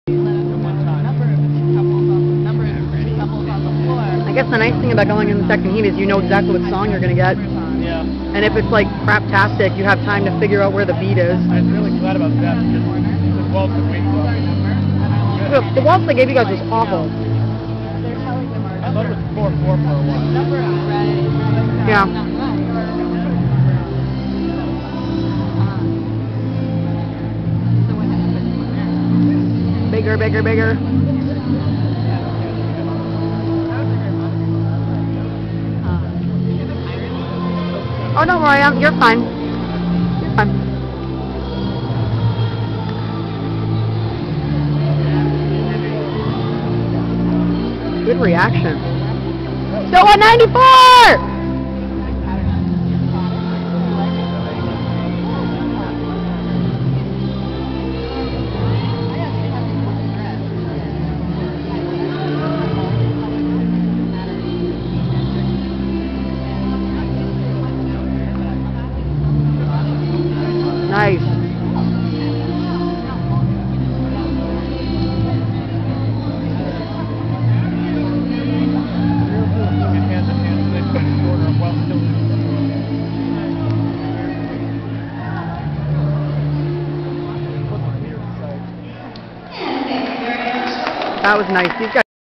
Tufts2012 WaltzBronze1b Am
American Bronze Waltz - 1st Round - David and Kristyne